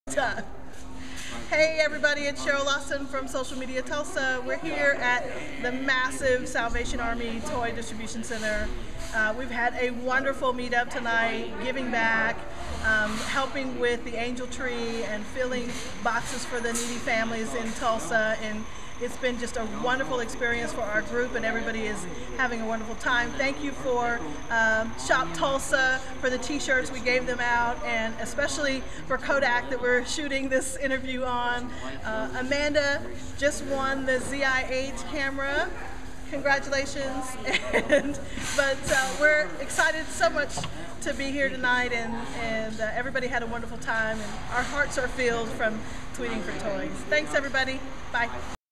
Tweet4toys Meetup Interview with Cheryl Lawson
Social Media Tulsa's December meetup at the Salvation Army Tulsa. Members donated toys for the Tweet4toys campaign as well as helped fulfill family baskets for the Angel Tree Program. Cheryl Lawson thanks Kodak, and Shop Tulsa who were among a few of the sponsors.